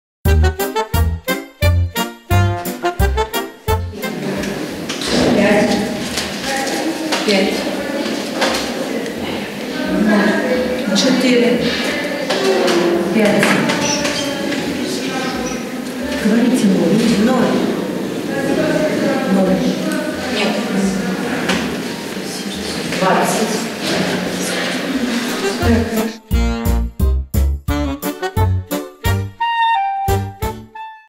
Урок матэматыкі ад выбарчай камісіі: 5+5+4+5=20 | Математика от избирательной комиссии
Як лічаць галасы на выбарчым участку №73, які разьмяшчаецца ў 201-й менскай школе.

Как считают голоса на избирательном участке №73, который размещается в 201-й минской школе.